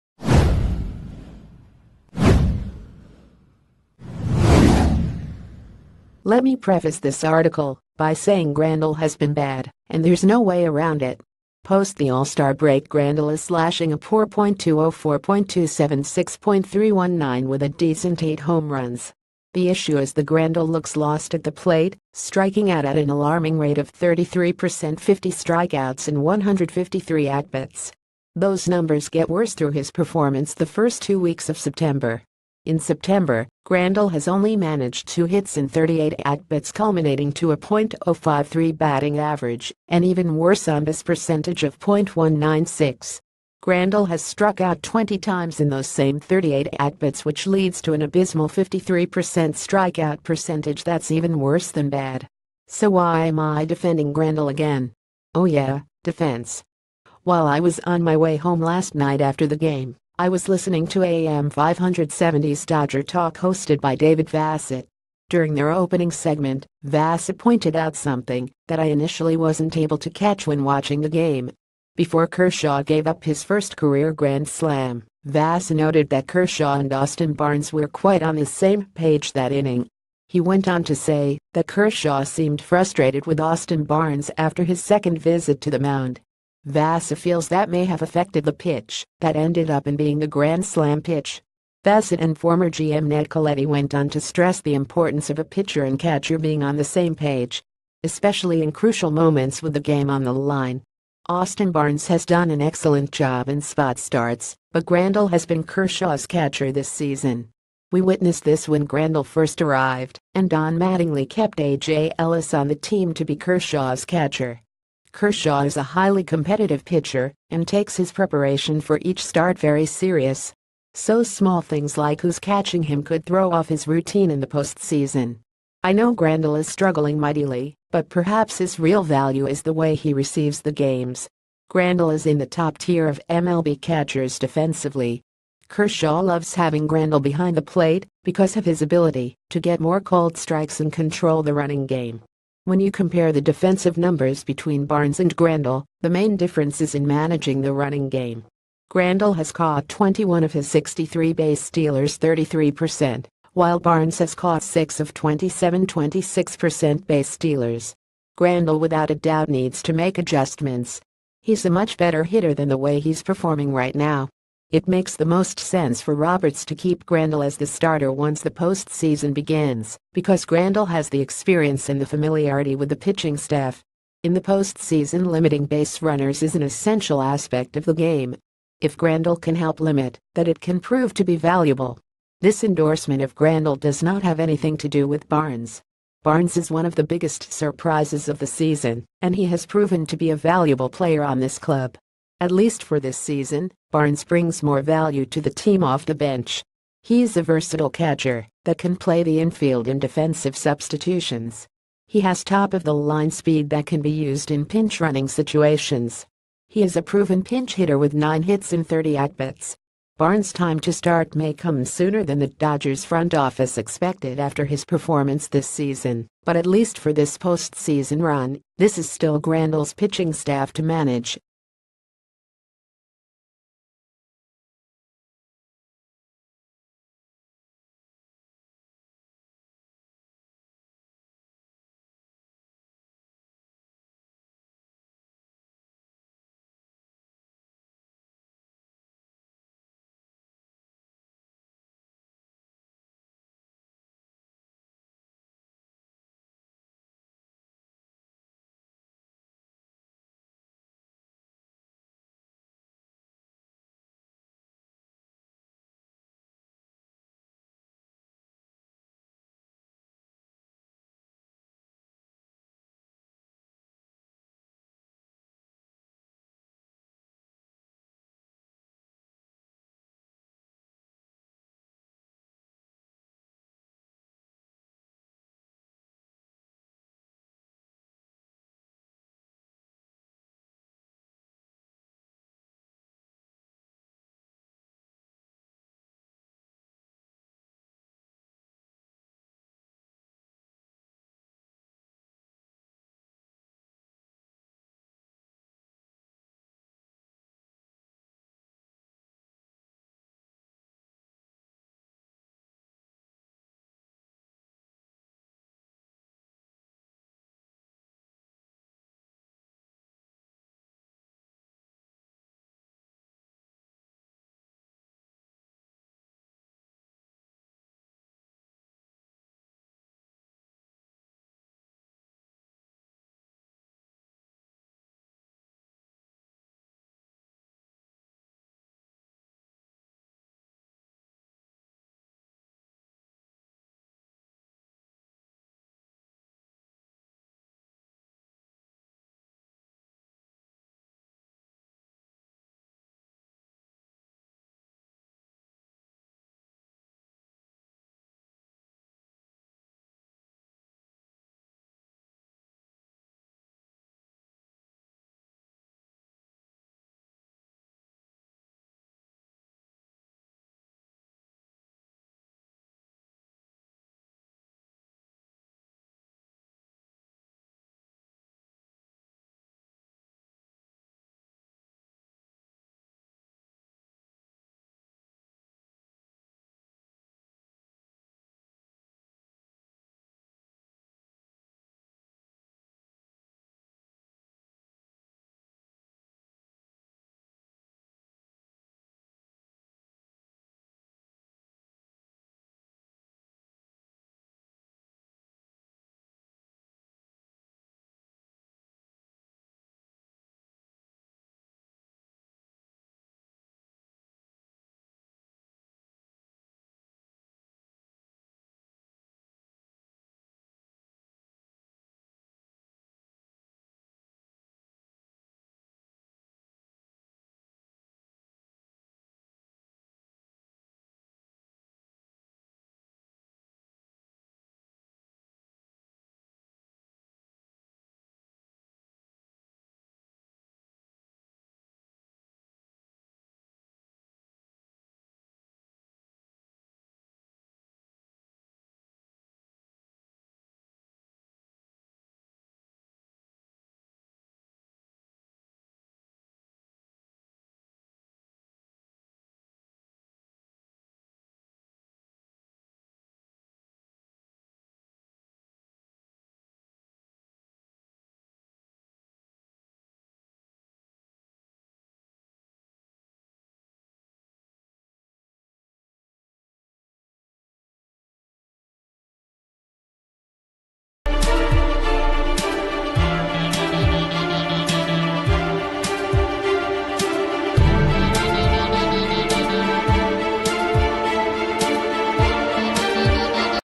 Breaking News | Yasmani grandal is the dodgers catcher for the postseason
Yasmani grandal is the dodgers catcher for the postseason

Let me preface this article by saying Grandal has been bad, and there’s no way around it. Post the A...